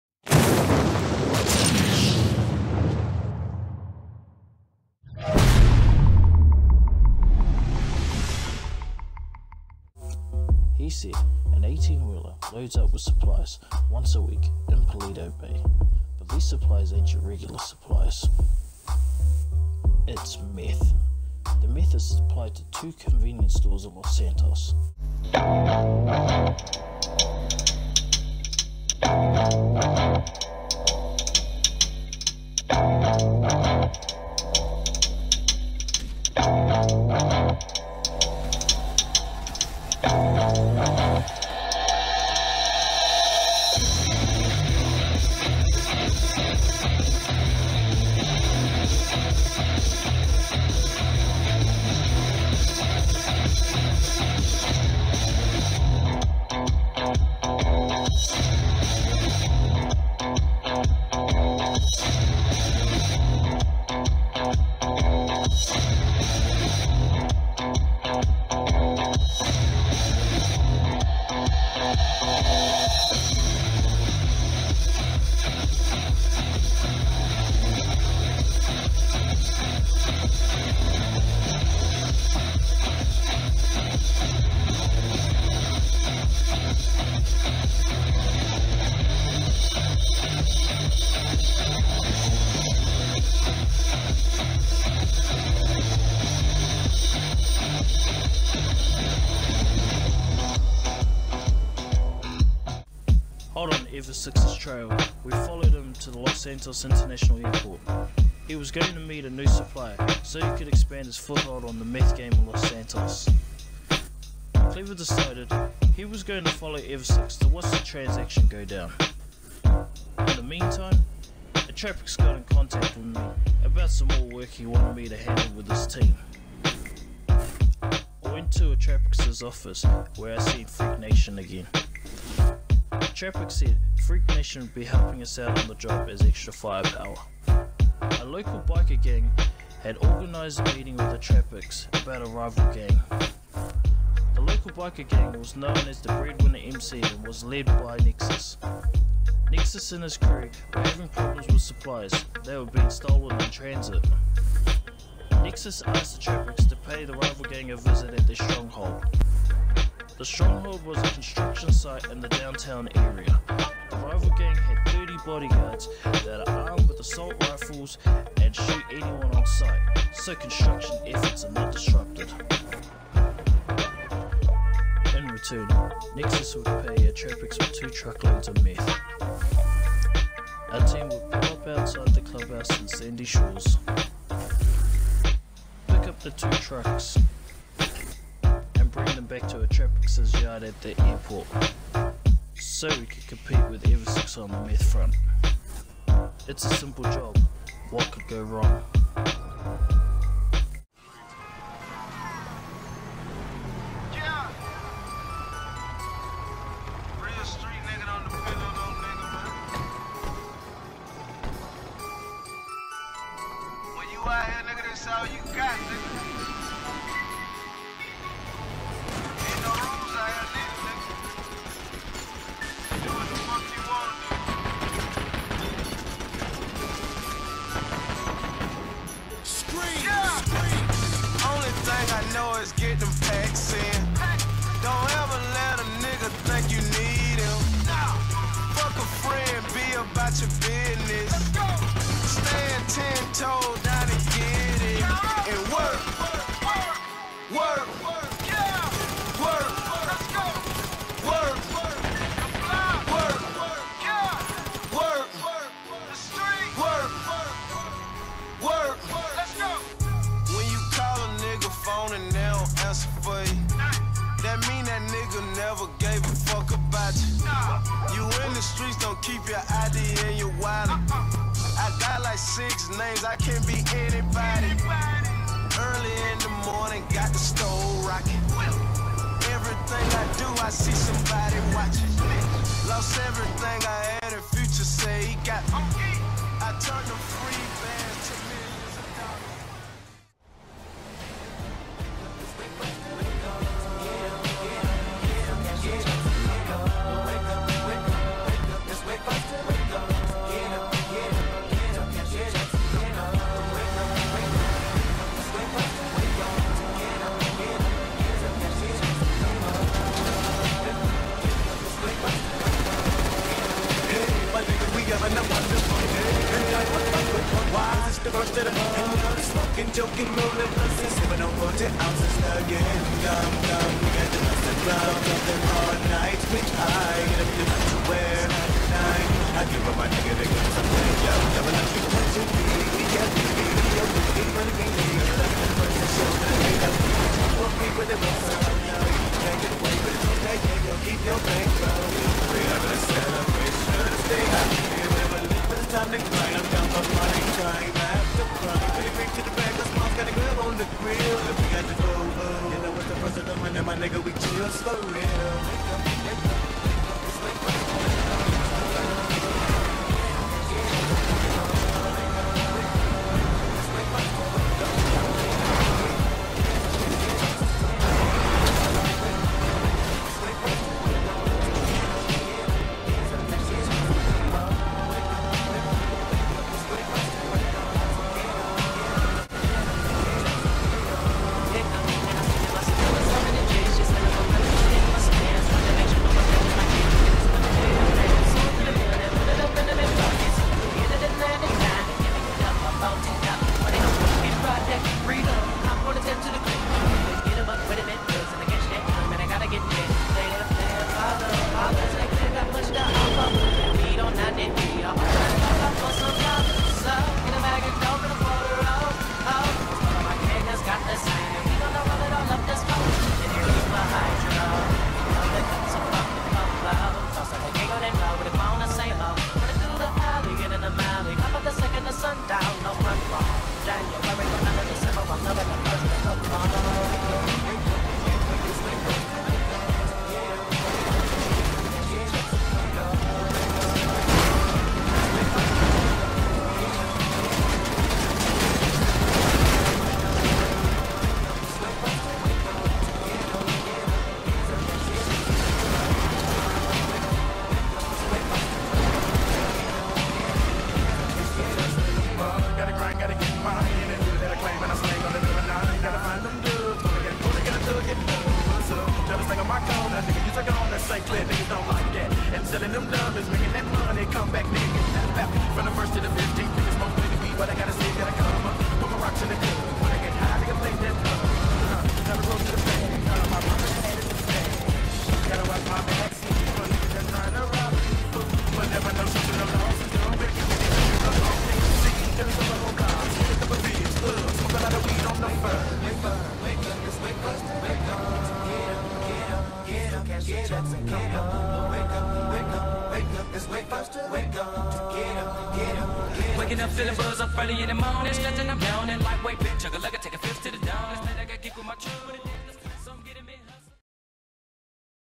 RAGS TO RICHES EP 3
Atrapix makes a bold move by buying a couple truck loads of methamphetamine. Meanwhile Eversix is traveling out of Los Santos to find a new supplier.